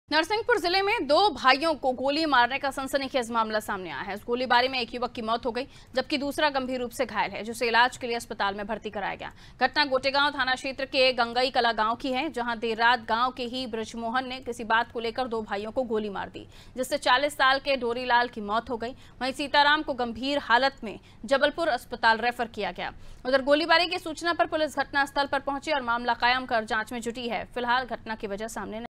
Narsinghpur : दो भाइयों को मारी गोली, एक युवक की मौत, एक गंभीर घायल || Anaadi Tv
नरसिंहपुर जिले में दो भाइयों को गोली मारने का सनसनीखेज मामला सामने आया है...इस गोलीबारी में एक युवक की मौत हो गई...जबकि दूसरा गंभीर रूप से घायल है...जिसे इलाज के लिए अस्पताल में भर्ती कराया गया है...घटना गोटेगांव थाना क्षेत्र के गंगई कला गांव की है...जहां देर रात गांव के ही ब्रजमोहन ने किसी बात को लेकर दो भाइयों को गोली मार दी...जिससे 40 साल के डोरीलाल की मौत हो गई...वहीं सीताराम को गंभीर हालत में जबलपुर अस्पताल रेफर किया गया।

AIRTEL - 379